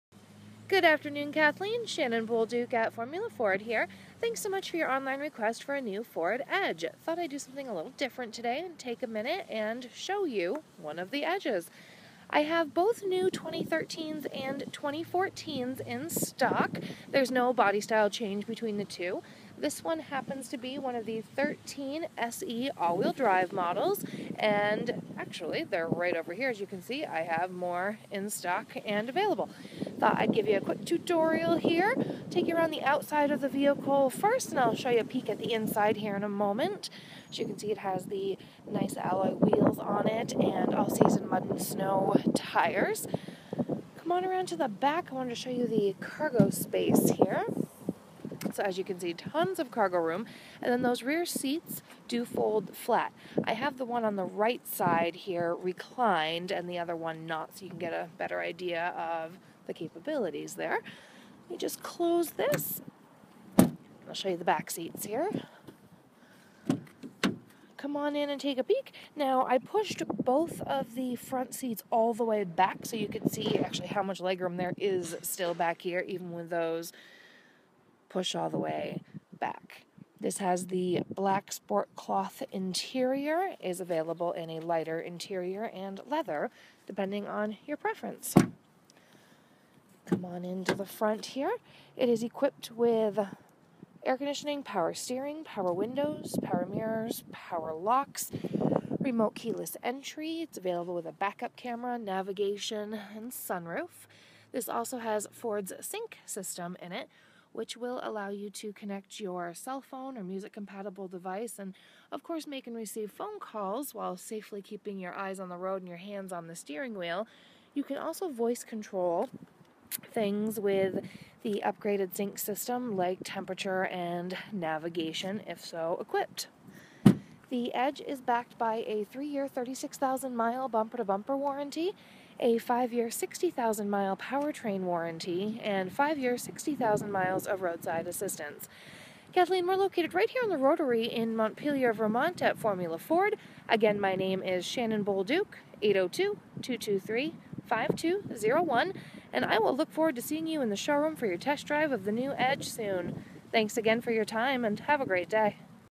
2013 Ford Edge SE AWD from Formula Ford for Kathleen!
2013 Ford Edge SE AWD from Formula Ford in Montpelier, Vermont. Featuring All Wheel Drive, Power Windows, Power Locks, Cruise Control, Remote Keyless Entry, Sync, and more!